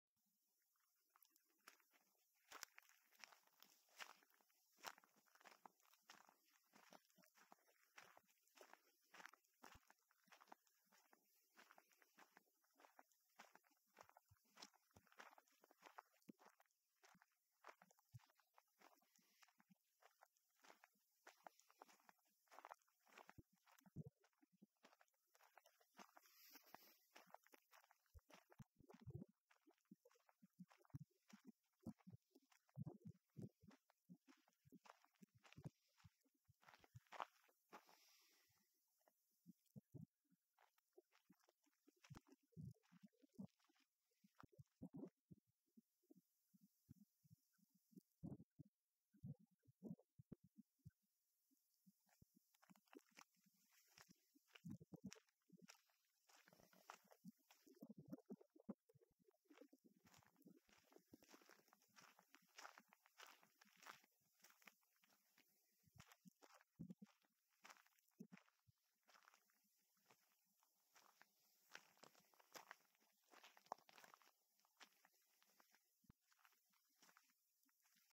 San Rafael Swell (Utah) | Little Grand Canyon | Wedge Overlook Campsite #2
Short clip of walking near the edge of the wedge overlook at the Little Grand Canyon.